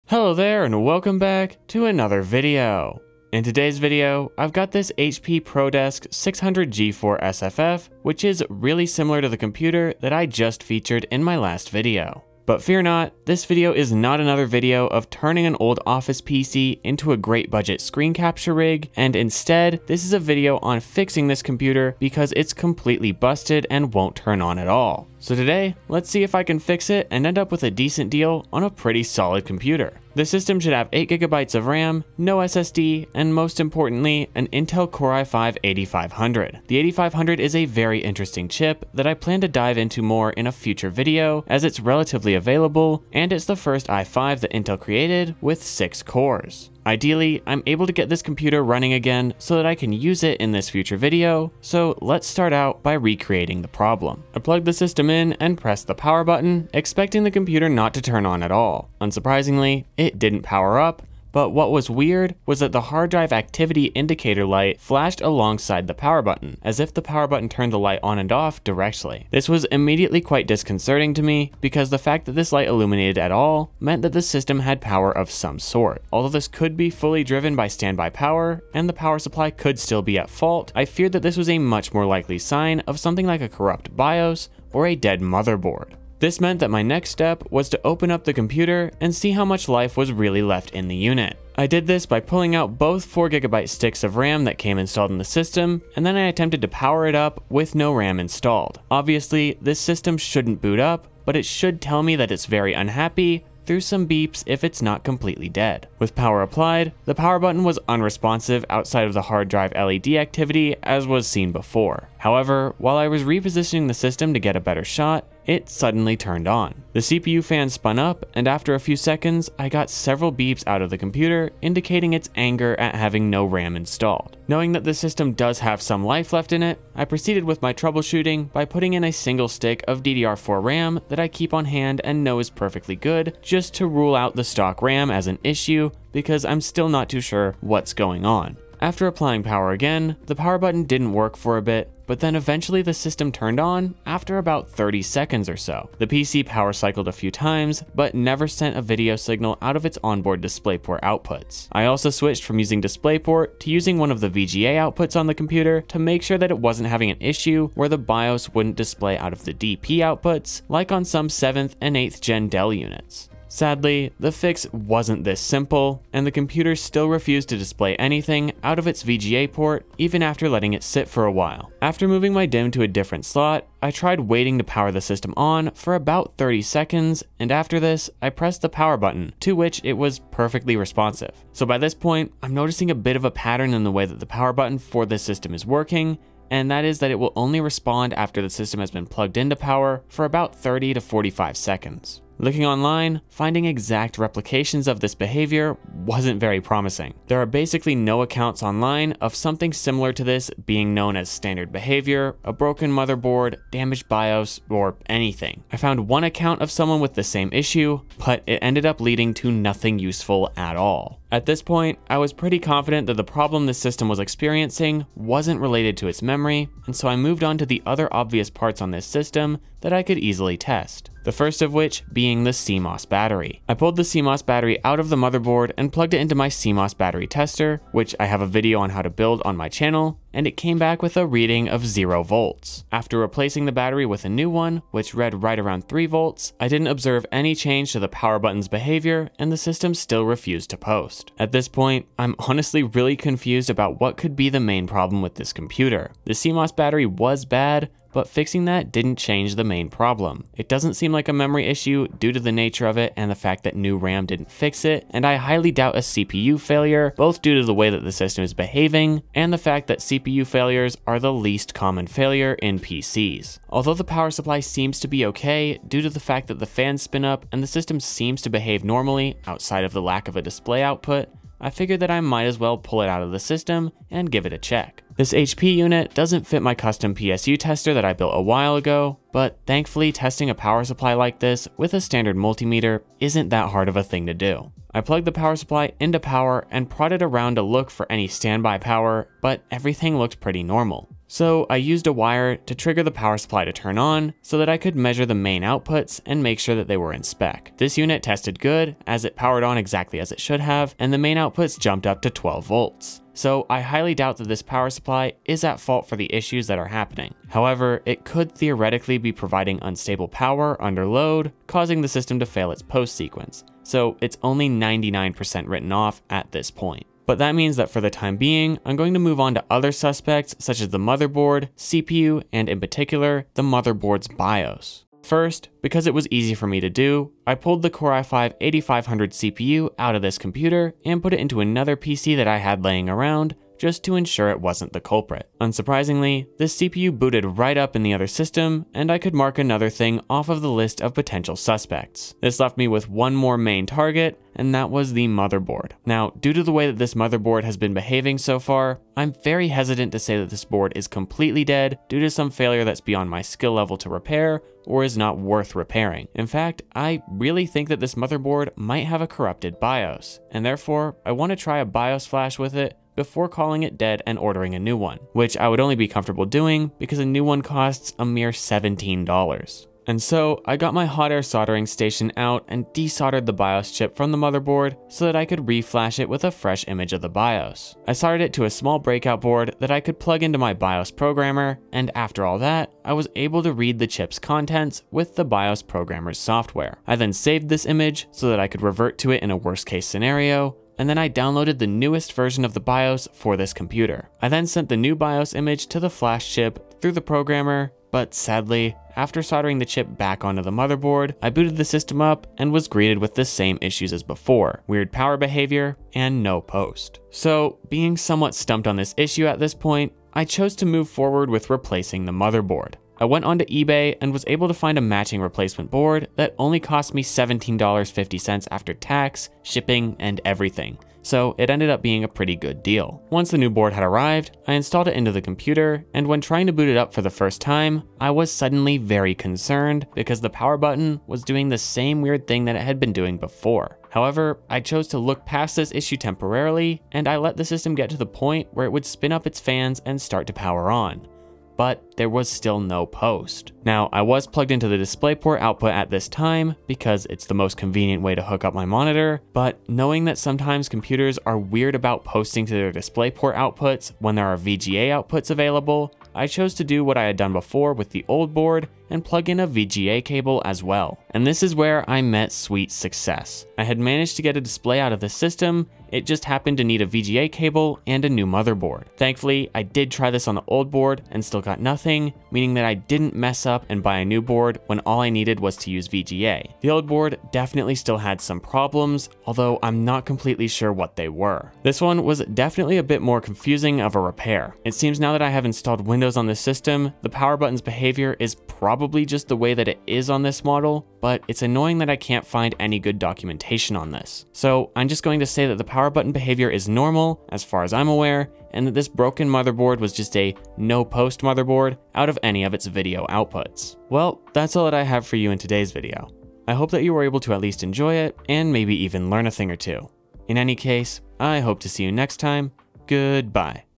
This $65 PC Won't Boot Up Properly - Let's Fix It!
Hello there and welcome to my video. In today's video, I'm going to be fixing an HP ProDesk 600 G4 SFF with an i5 8500 that won't power on normally or POST. This PC's problem was a little more convoluted than I originally thought, but thankfully this didn't stop me from getting it working in the end.